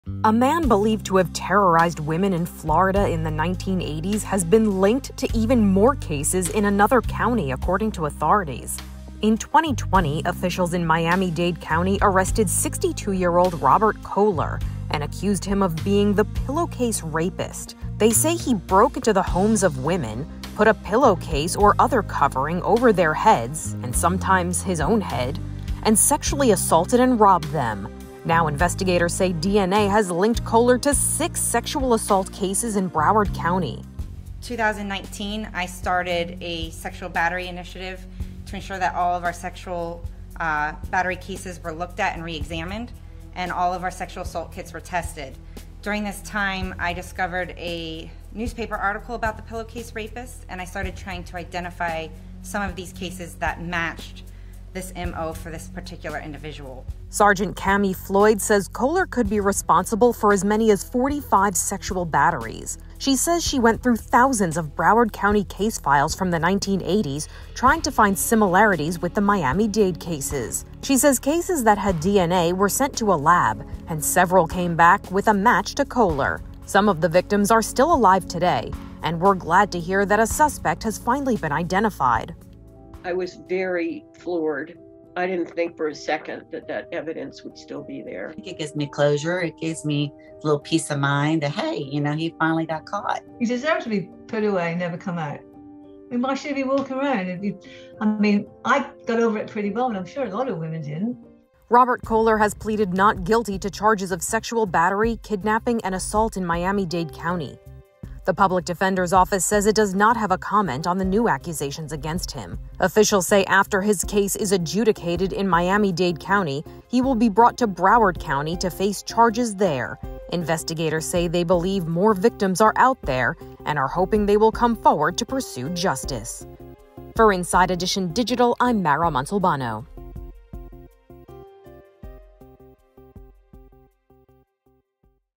6 More Cases Linked to ‘Pillowcase Rapist’ in Florida: Cops
A man accused of terrorizing women in Florida in the 1980s has been linked to more cases in another county. In 2020, officials in Miami-Dade County arrested Robert Koehler, 62, and accused him of being the Pillowcase Rapist. They say he broke into homes of women, put a pillowcase or other covering over their heads and sometimes his own head, and sexually assaulted them. Investigators say DNA linked Koehler to six more cases in Broward County. Inside Edition Digital’s Mara Montalbano has more.